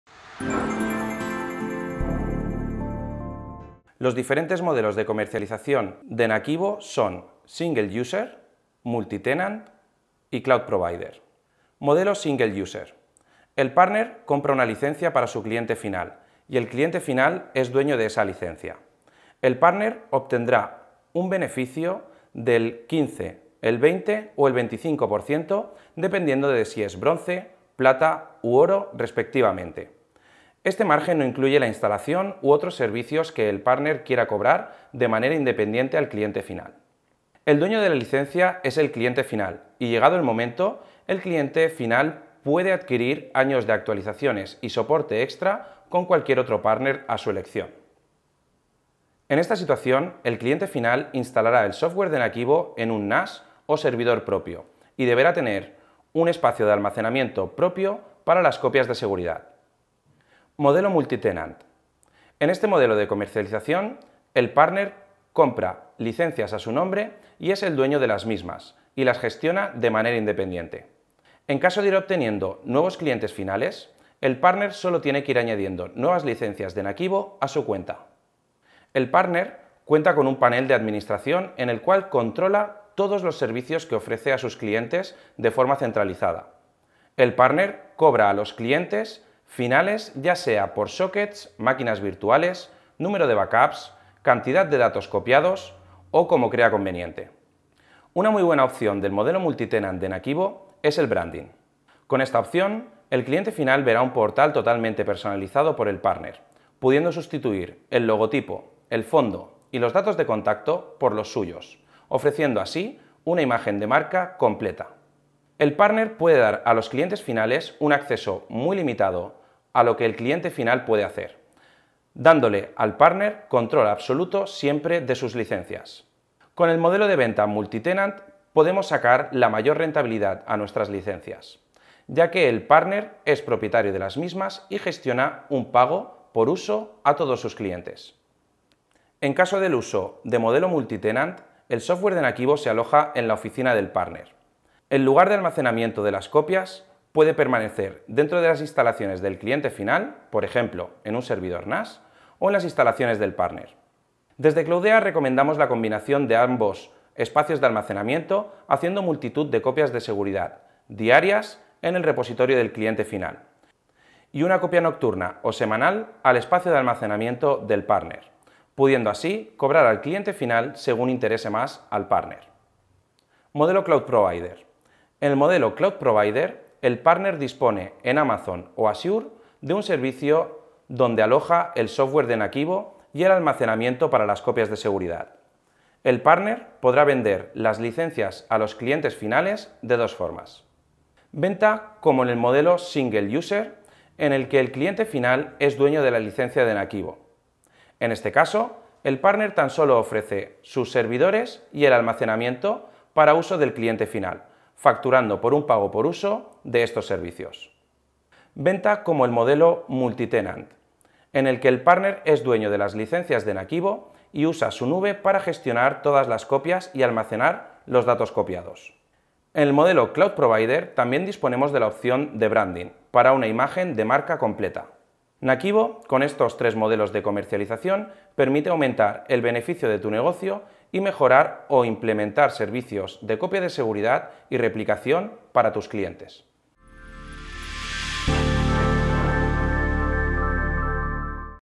Modelos de Venta NAKIVO - Licencias Multitenant y Cloud Provider
En tan sólo 4 minutos descubrirás los distintos modelos de comercialización que seguro se adaptarán a las necesidades de tus clientes.

Estos modelos de comercialización de NAKIVO son:
Single User: El partner compra una licencia para su cliente final y el cliente final es dueño de esa licencia.
Multitenant: En éste modelo de comercialización, el partner compra licencias a su nombre, es dueño de las mismas y las gestiona de manera independiente.

Cloud Provider: El partner dispone en Amazon o Azure de un servicio donde aloja el software de NAKIVO y donde además ofrece el almacenamiento para las copias de seguridad.